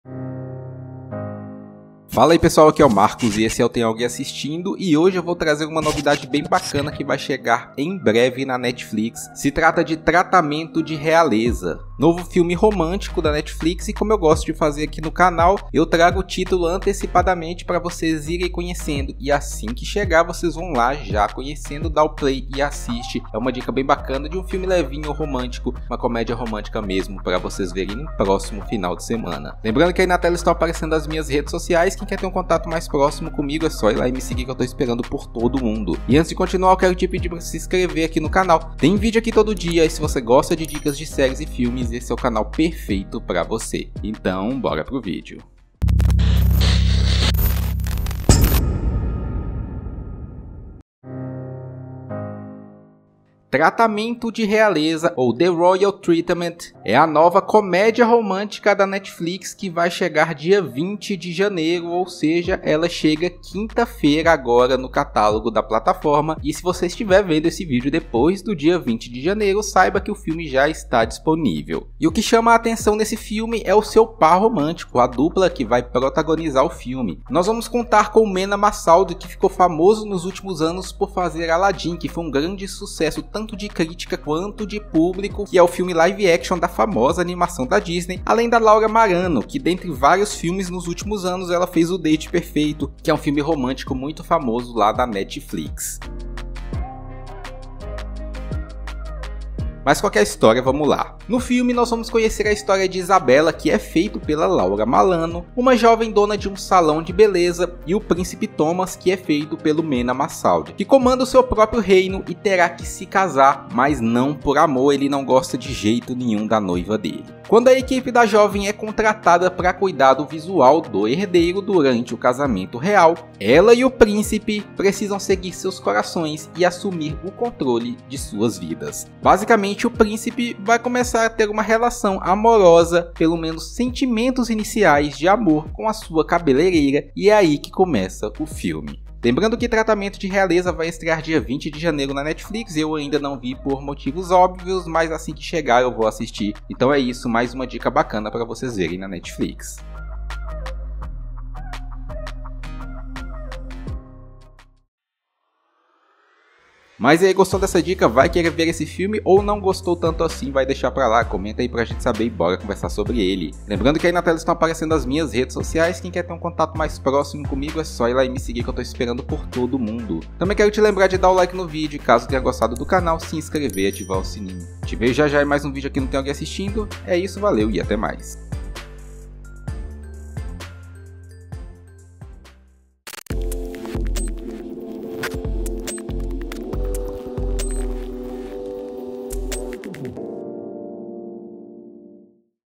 CONHEÇA O NOVO ROMANCE APAIXONANTE DA NETFLIX COM ATOR DE ALADDIN E A LAURA MARANO
Tratamento de Realeza é o novo filme de romance da Netflix e ele promete conquistar os fãs do gênero. Vem conhecer!

Seja muito bem vendo (a) ao canal do site Tem Alguém Assistindo?.